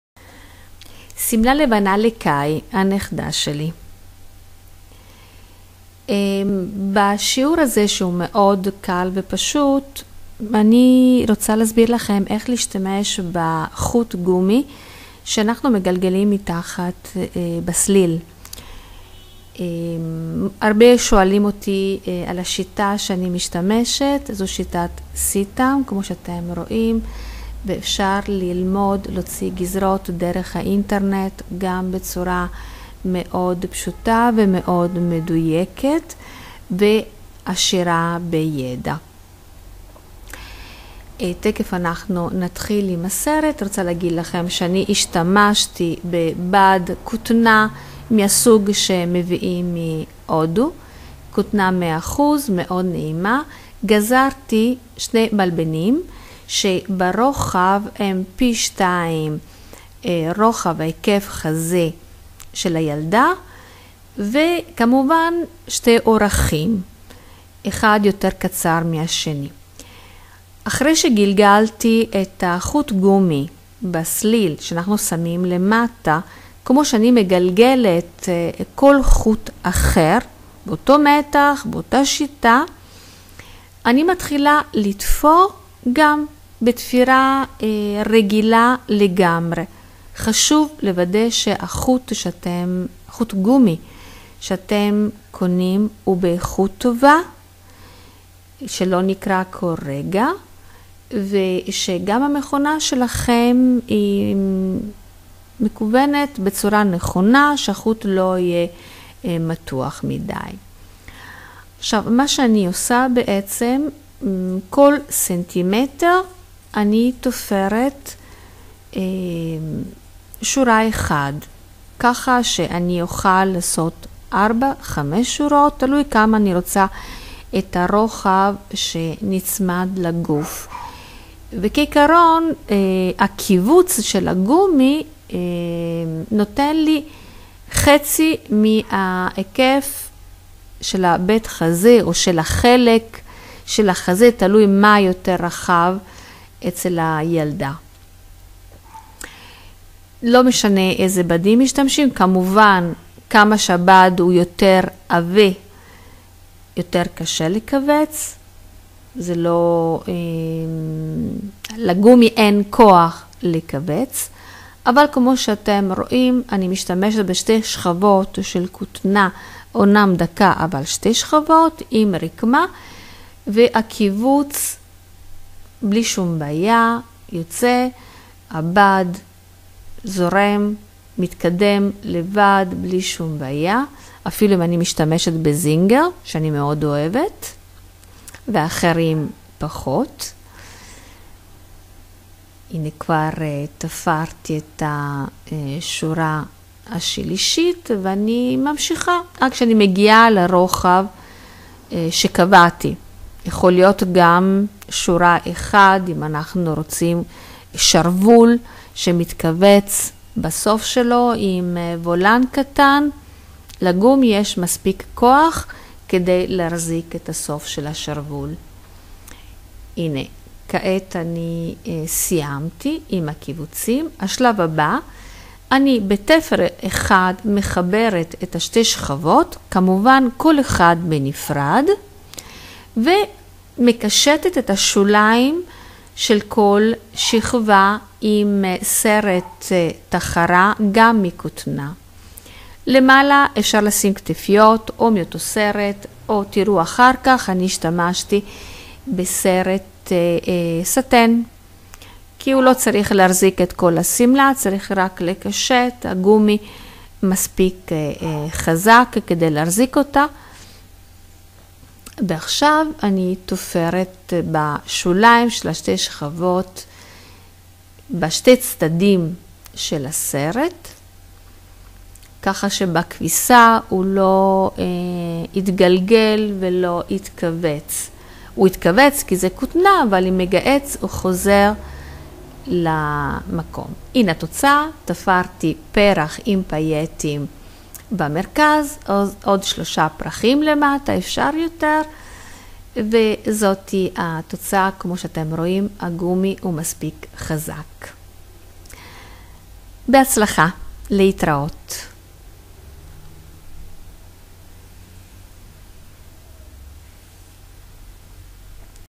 תפירה חוט גומי: l קורס תפירה ותדמיתנות SITAM-ITALIA
שימלה לבנה לילדה מכותנה עם חוט גומי שמכווץ בחזה